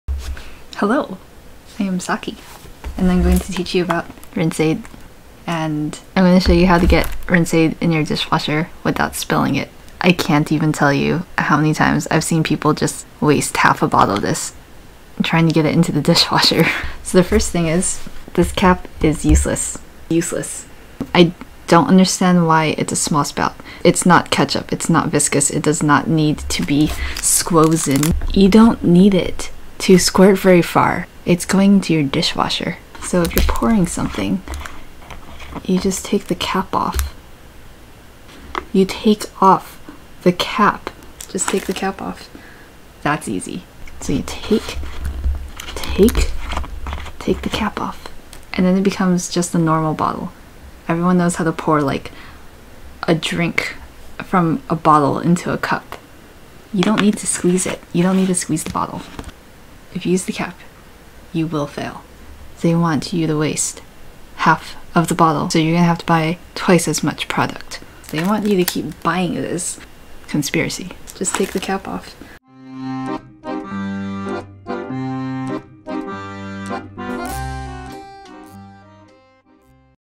How to Pour Rinse Aid without Spilling
Don't be a dishwasher n00b.

This tutorial shows you how to pour it in.